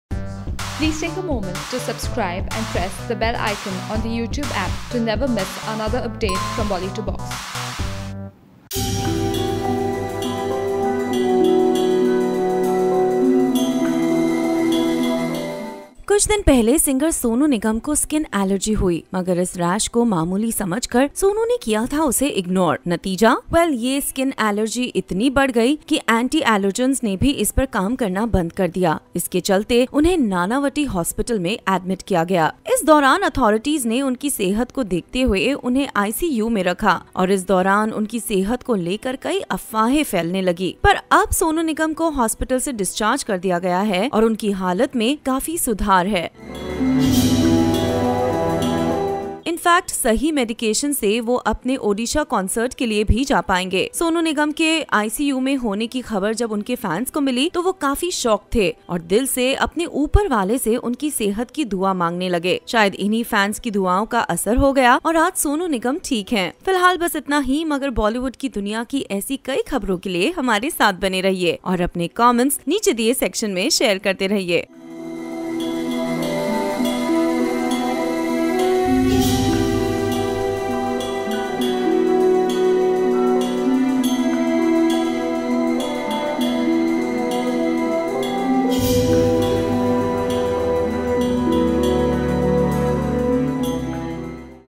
Sonu Nigam को गंभीर Skin Allergy के कारण Nanavati Hospital में किया भर्ती
SonuNigam को गंभीर SkinAllergy के कारण Nanavati Hospital में किया भर्ती